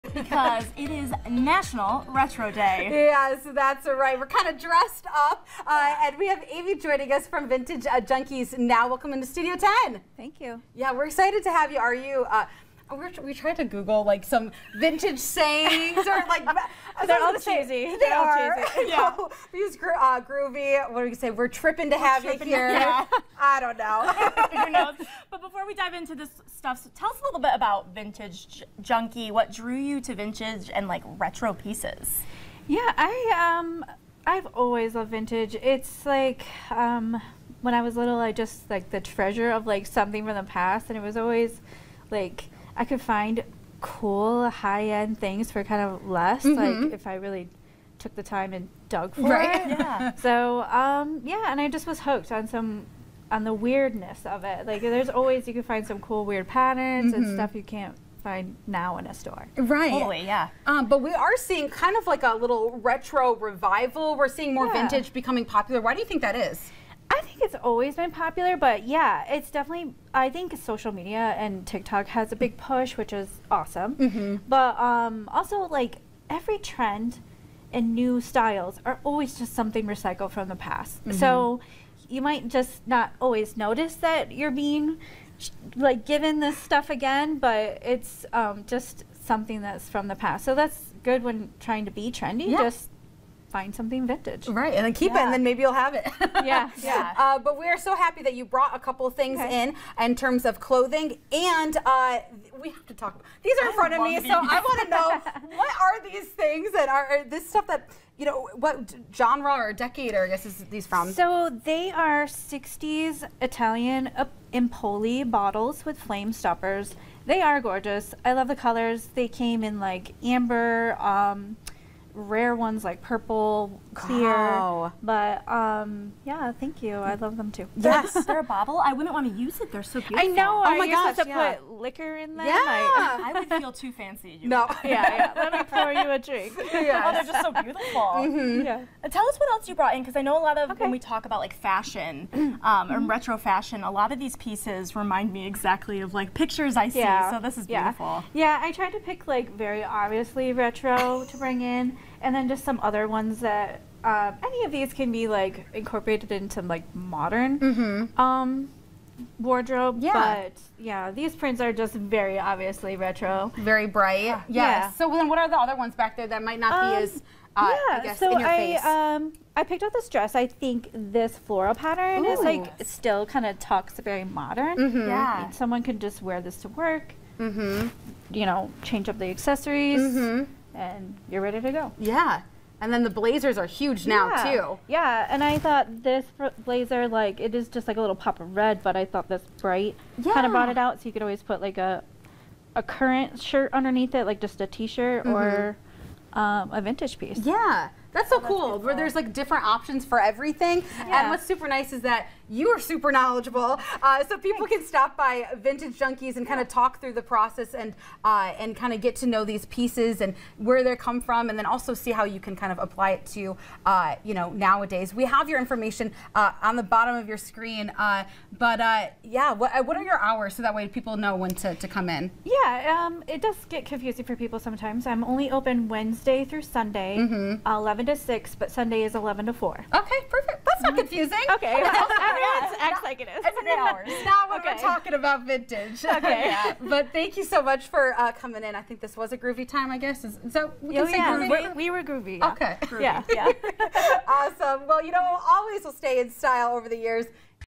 Turn Back Time
Hit rewind, it's National Retro Day! Vintage Junkies joins us to turn back time and find the treasures of the past. We also explore why old styles, from fashion to home decor, are seeing a revival!